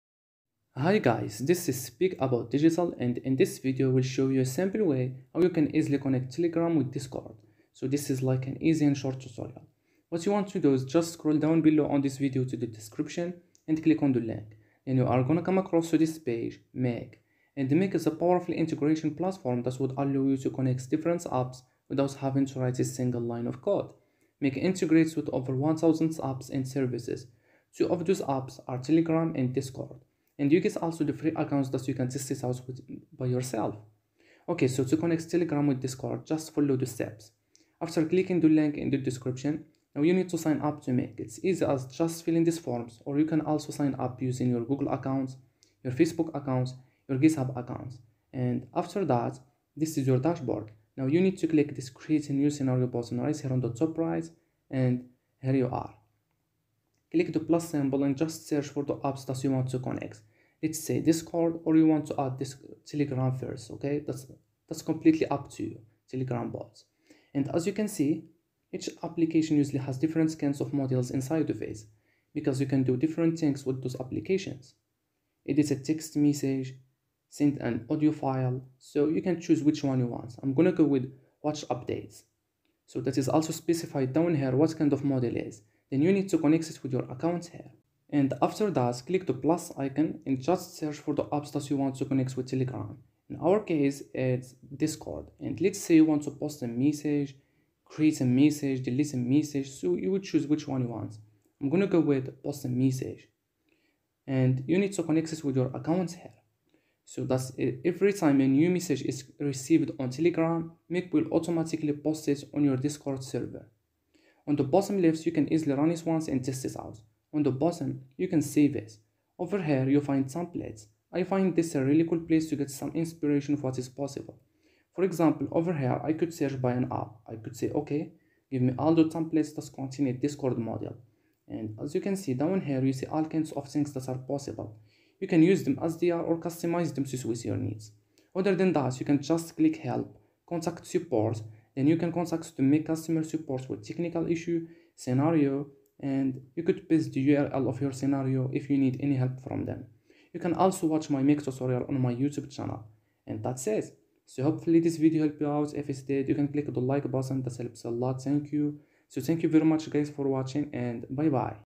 How To Integrate Telegram with Discord
🔥 Special Offers 🔥

In this video you will find out How To Connect Telegram to Discord